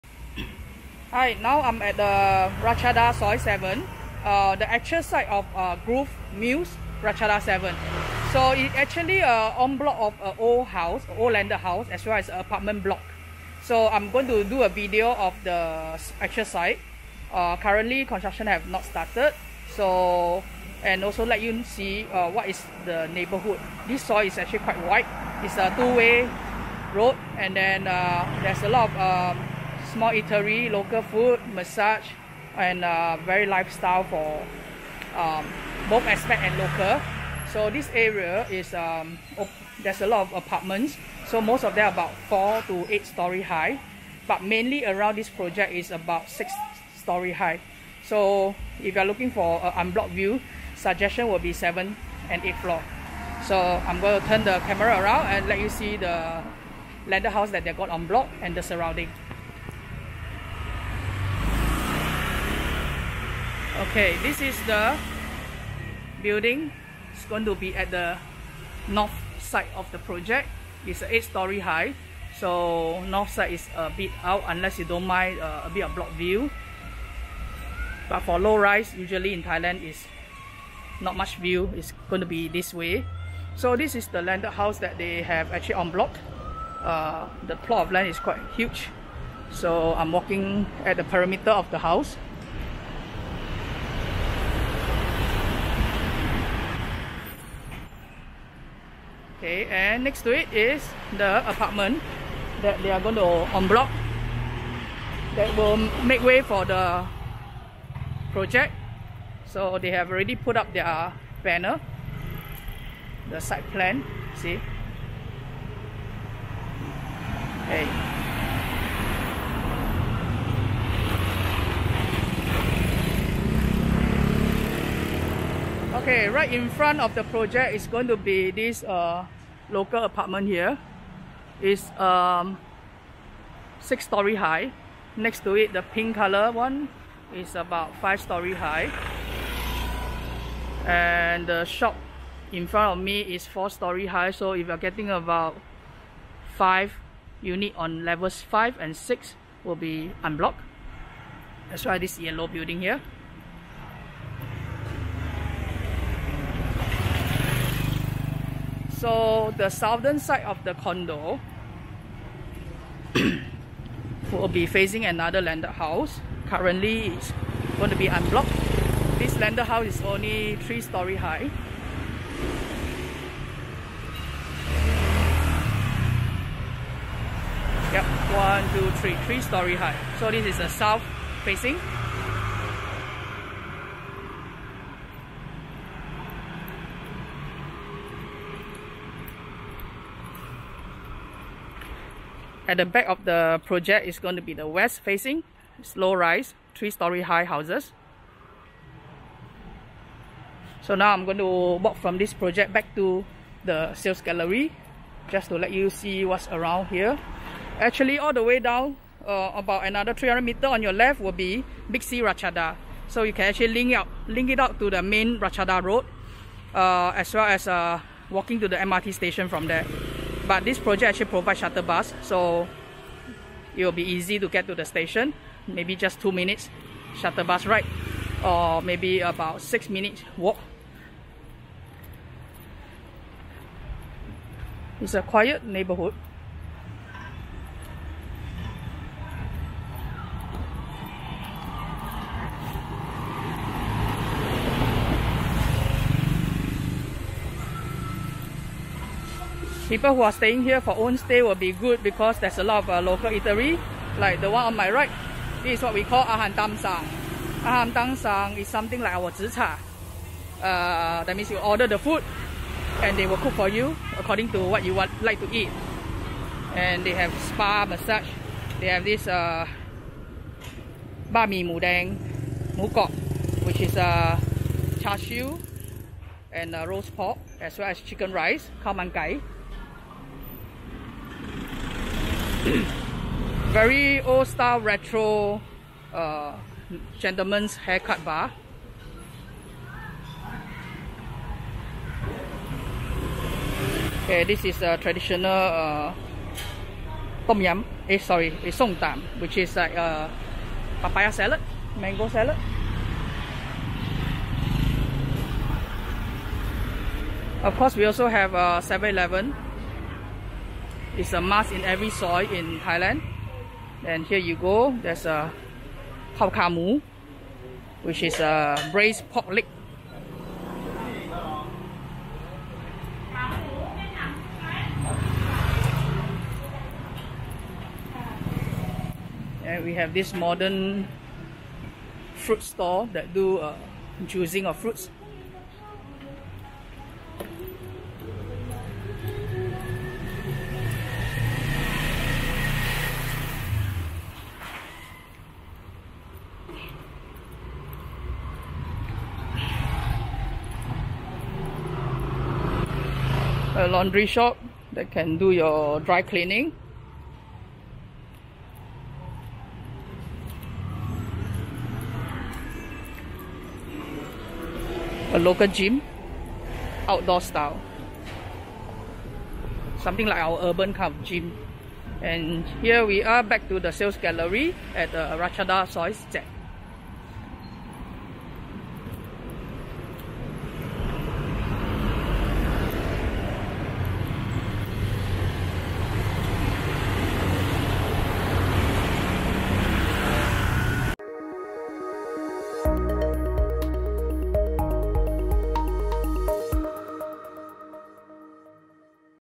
Groove Muse Ratchada 7 Actual Site Visit | Soi Ratchadapisek 7
Groove Muse Ratchada 7 is a new freehold development by Divine Development Holdings. It is near to The Street Ratacha and Big C Extra Ratchadapisek. The project is primarily aimed towards the young Thai homeowners with their relatively Low entry price point.

Get updated on all the latest Promotions And Launches